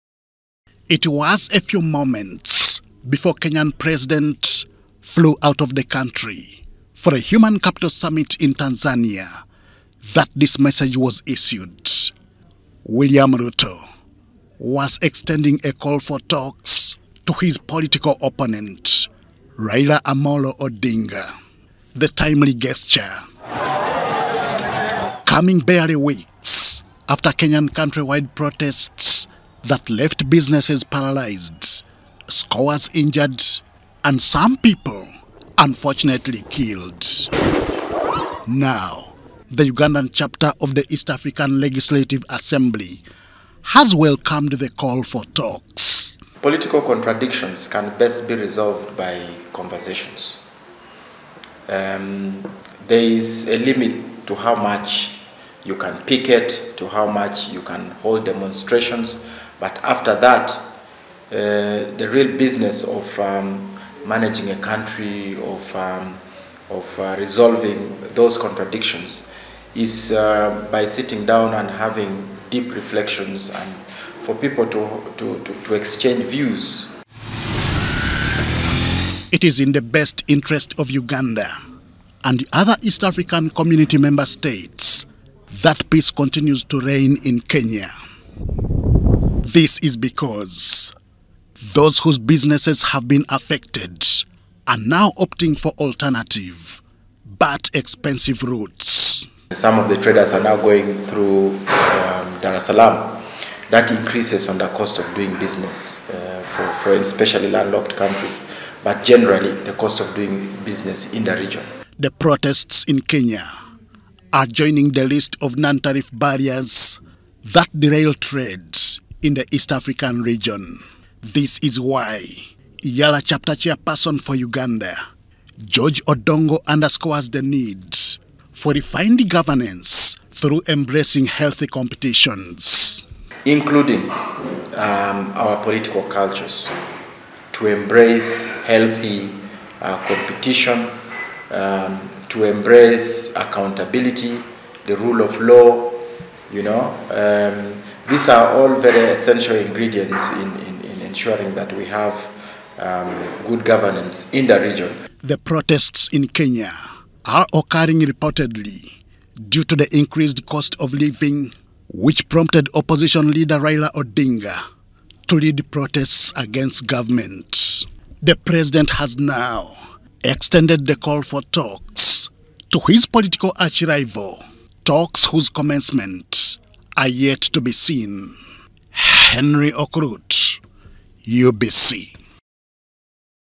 EALA - UGANDA CHAPTER CHAIRPERSON CALLS FOR DIALOGUE IN KENYA
The East African Legislative Assembly Chapter of Uganda is welcoming the call for dialogue between opposition and the government of Kenya.
The call follows recent protests against high cost of living that led to property destruction, human injuries and loss of lives.
The Uganda EALA Chapter Chairperson George Odongo has resounded the need for talks citing disrupted trade between Kenya and other East African Countries